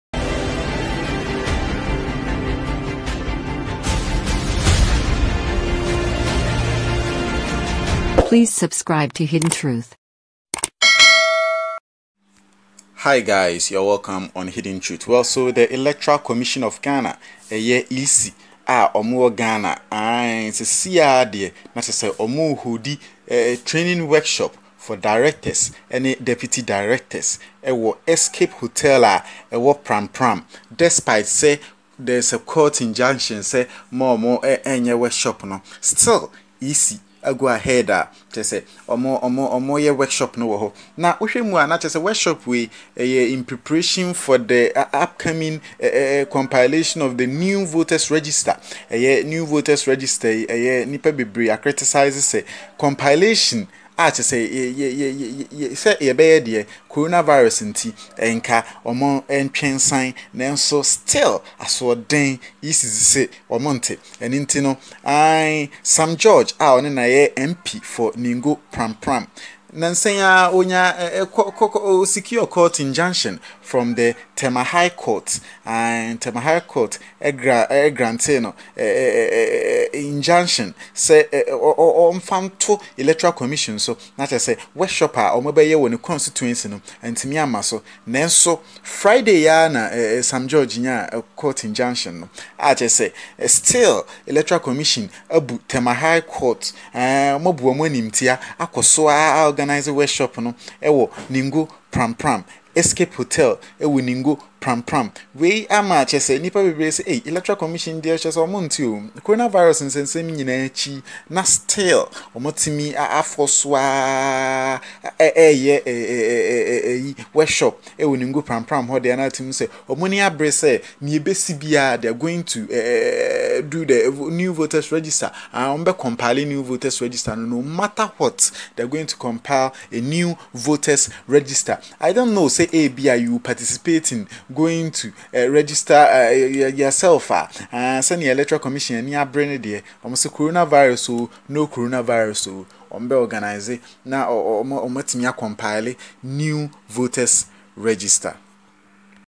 New voters register to come off despite outbreak [COMMENTARY]
EC goes ahead with training workshop despite court injunction